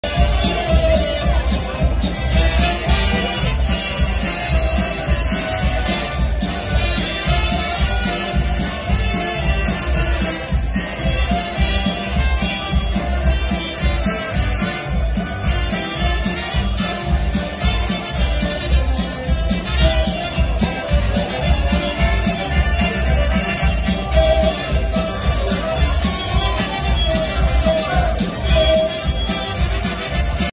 Pennsylvania Polkafest: 4/29/23
Enthusiastic dancers hit the floor to the sounds of John Stanky and the Coalminers during Saturday's Pennsylvania Polkafest inside the Genetti's Grand Ballroom.